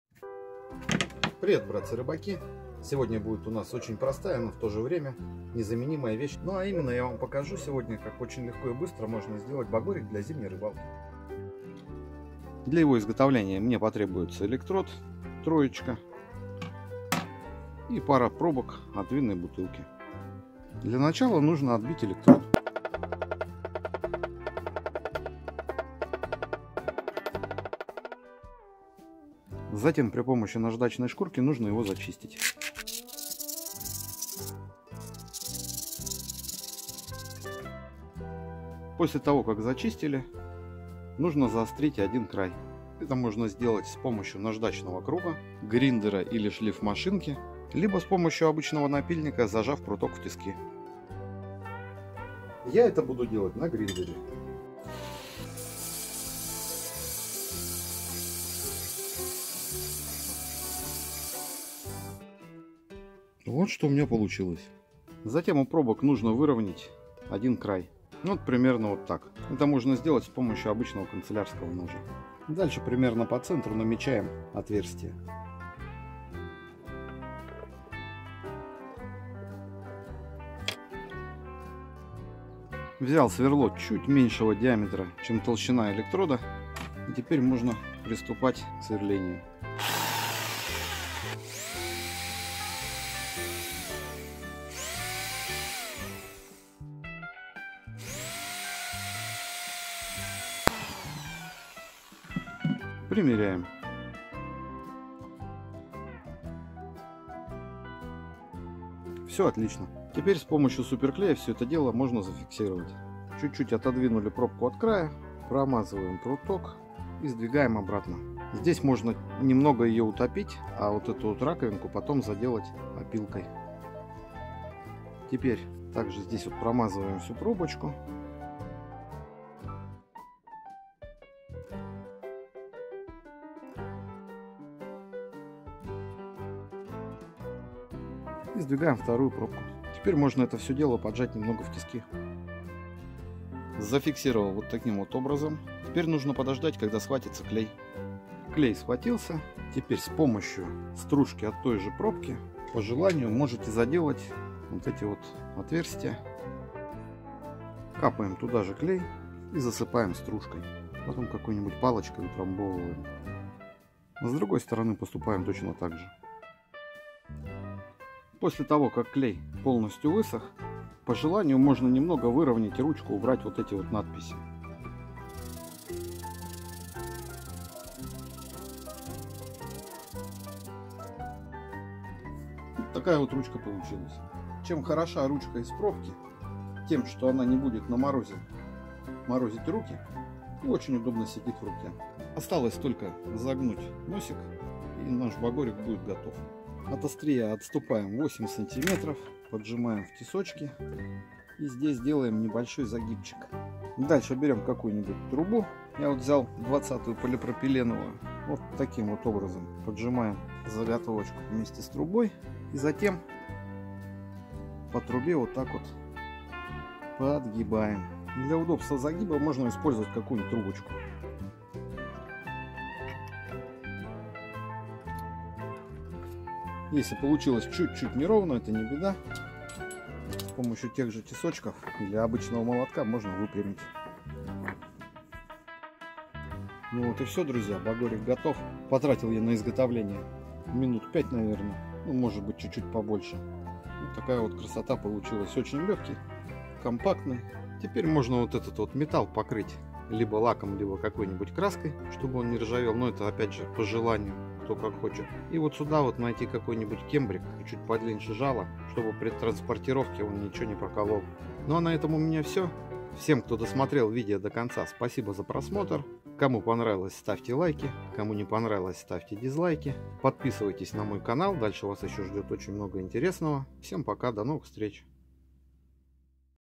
Багорик для щуки своими руками.
Очень простой и быстрый способ изготовления багорика. Незаменимый инструмент для ловли щуки зимой!